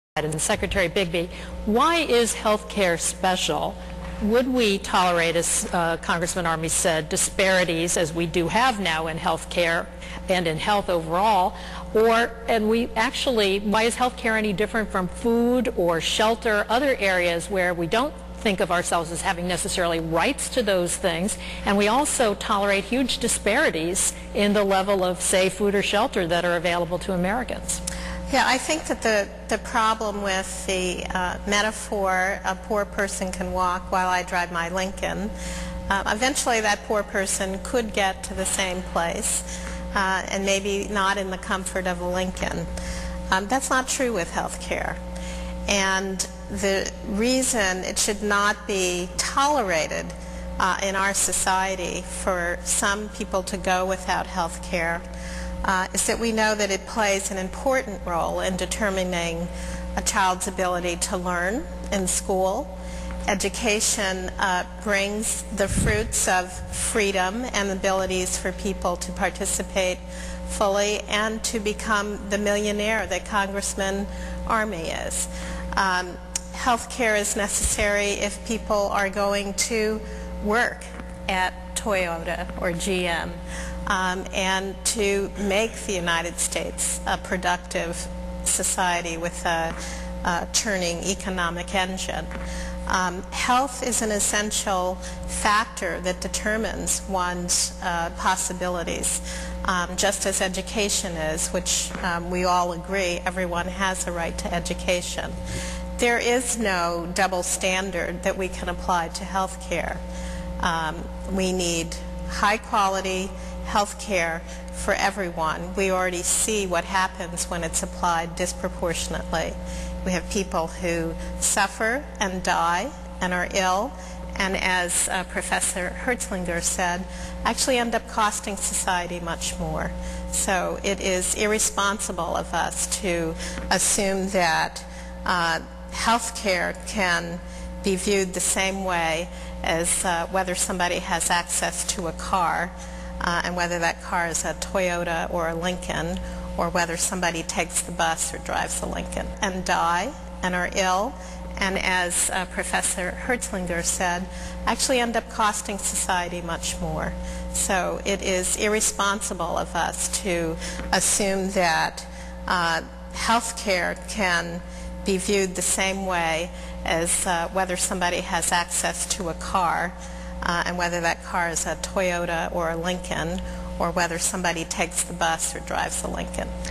Why is health care different from food or shelter?
Bigby says we should not tolerate people living without health care because it impacts the ability of individuals to become productive members of society.  Health is a key factor that determines one's possibilities in life.  We need high-quality health care for everyone.